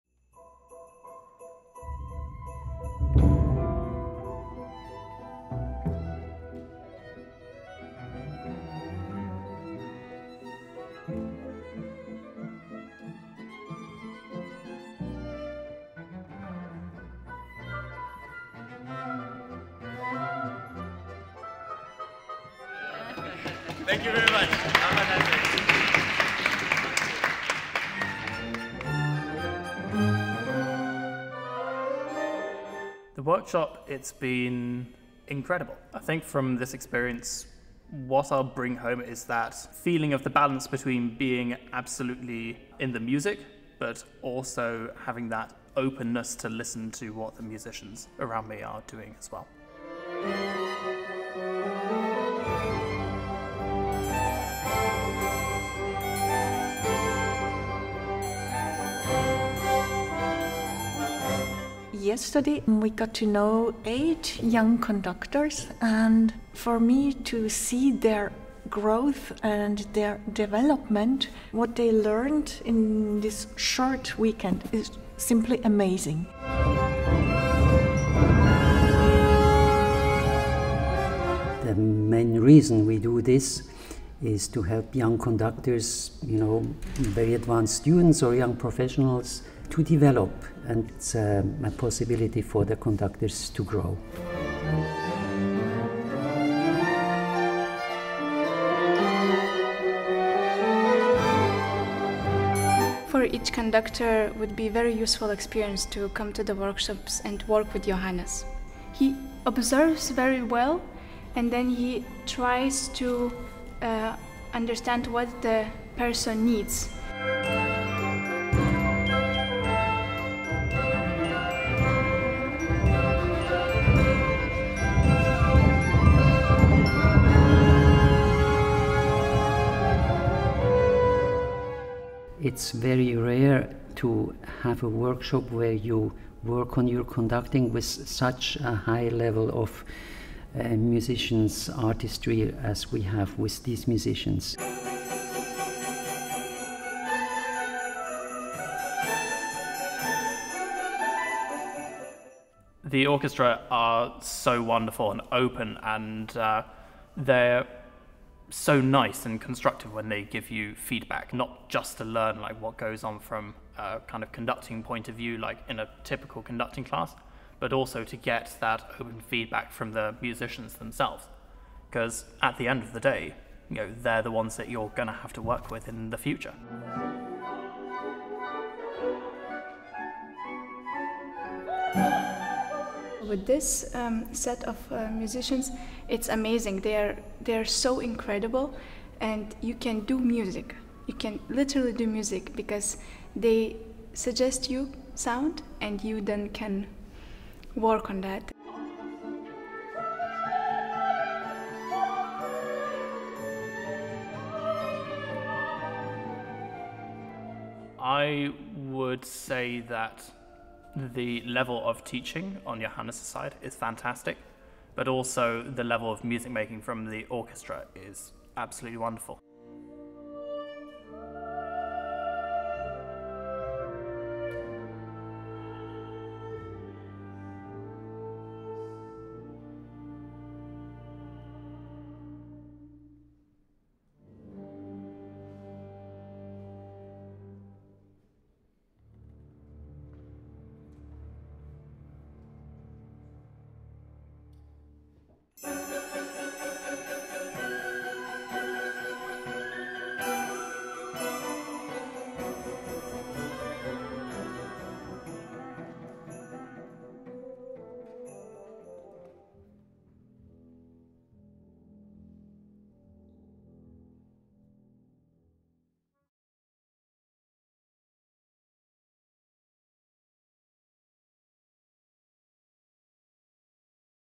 Masterclass Basel 2022 - Johannes Schlaefli Conducting Academy
Trailer aoubt first master-class of "Conducting Academy Johannes Schlaefli" with Kammerorchester Basel October 2022